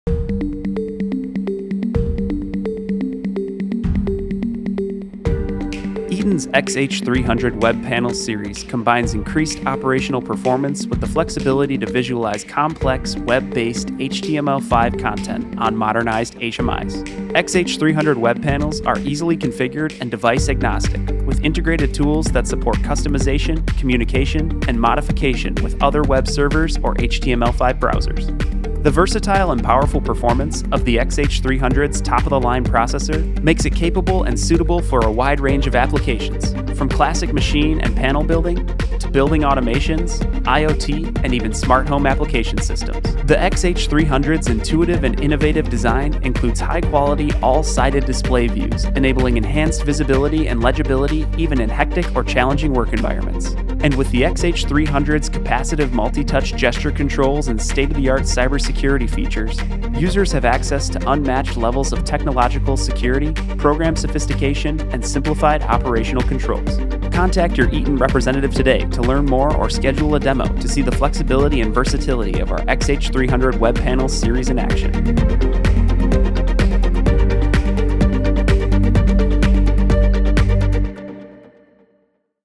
Eaton's XH300 web panel series | Increased operational performance with web-based HTML 5 content
Learn how the XH300 web panels offer integrated tools that support customization, communication and modification with other web servers or HTML 5 browsers